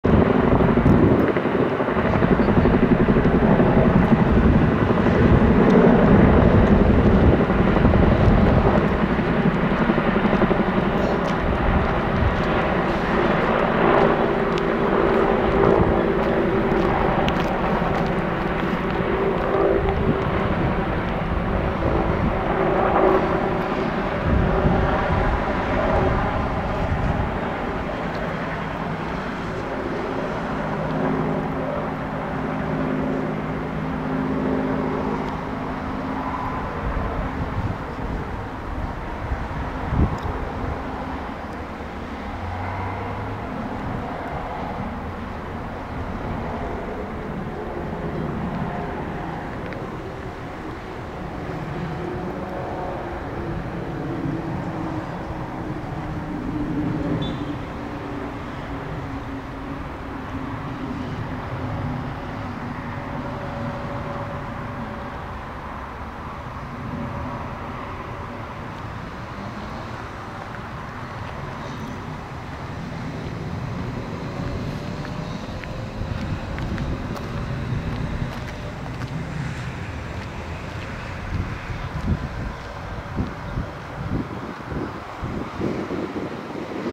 Short impression military Operation Falcon Leap with 2x Chinook 1x Apache (16-09-2021)
Defense is practicing with Operation Falcon Leap with the dropping of soldiers and cargo loads in various places above Gelderland, the north of the Netherlands and parts of Belgium. This short impression shows how several international soldiers jump over Houtdorperveld, the Netherlands in preparation for the annual commemoration of Operation Market Garden, which will take place a little later on the Ginkelse Heide.

Dotwood Media 🔜🎥